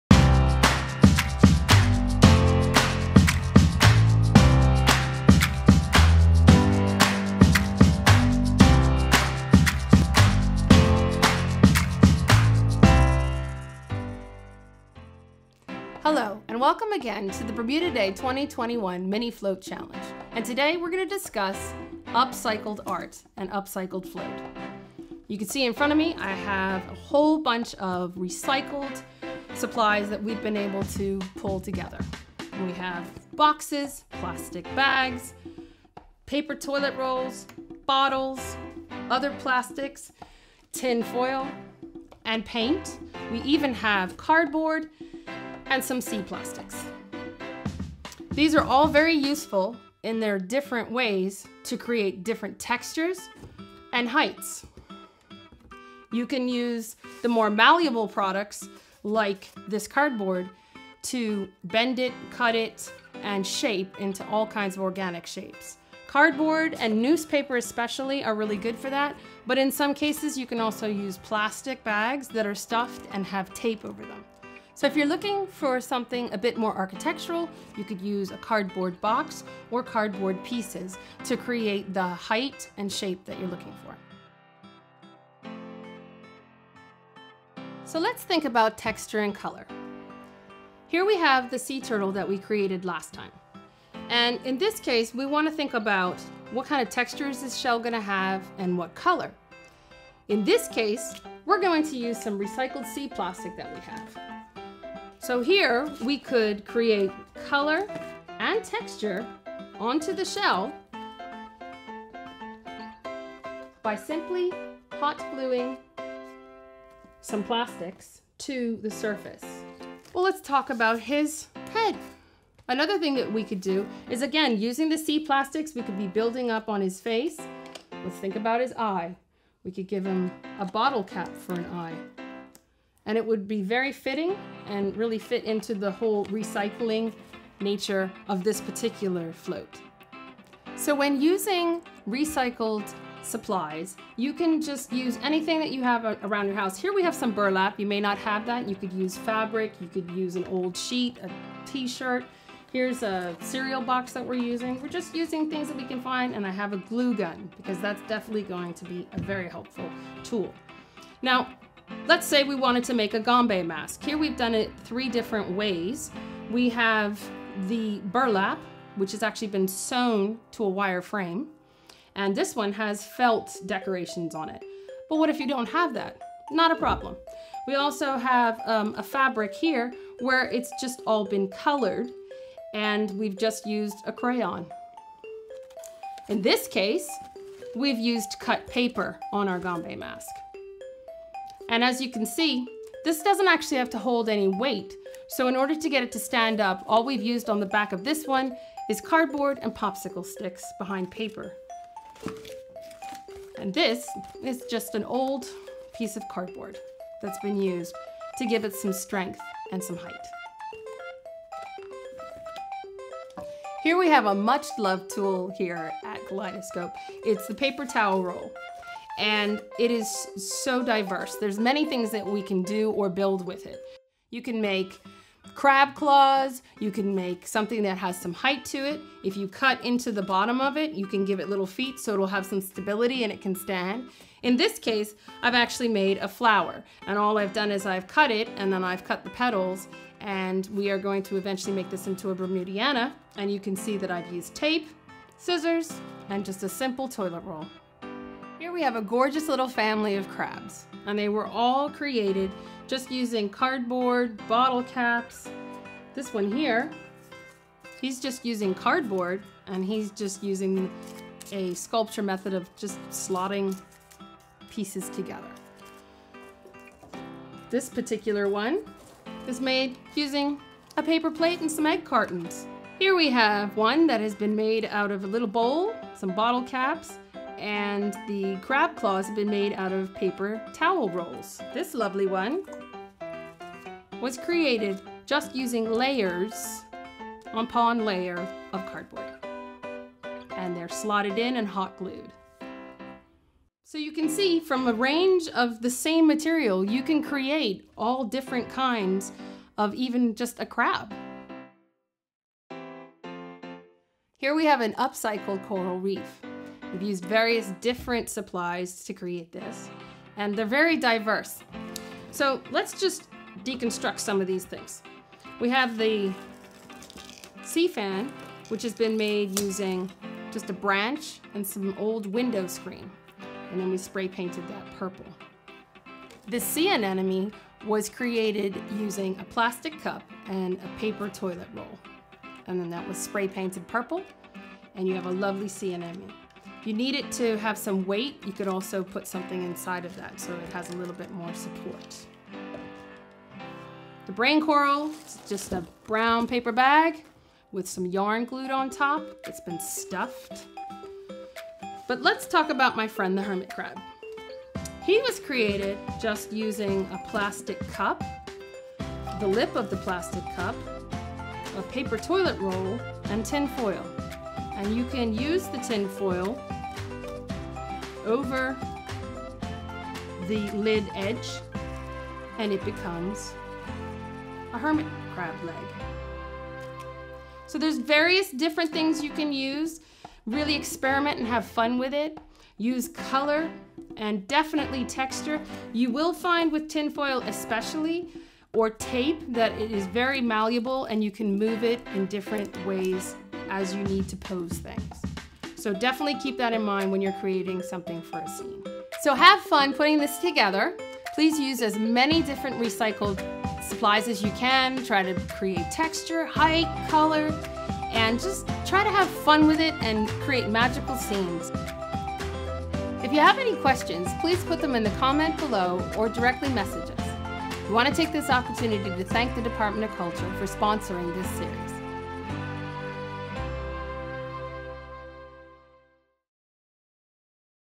Mini Float Challenge 2021: Using Upcycled Materials
Corrina Rego at Kaleidoscope Arts Foundation gives a tutorial on how to use upcycled materials to decorate your entry for the Heritage Month Mini-Float Challenge.